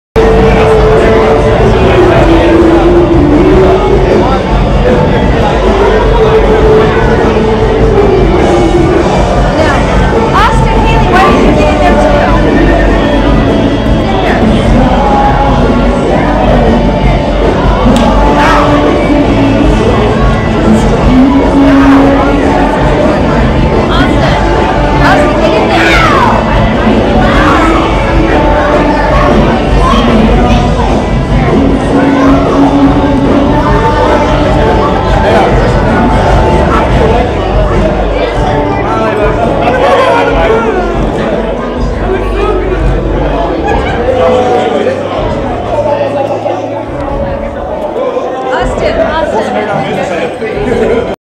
Kenton Duty and Davis Cleveland at IPOP with Joseph Beauchamp
Kenton Duty and Davis Cleveland from Disney Channel's Shake It Up on the red carpet at the IPOP National Convention in Los Angeles, California.

Joseph Beauchamp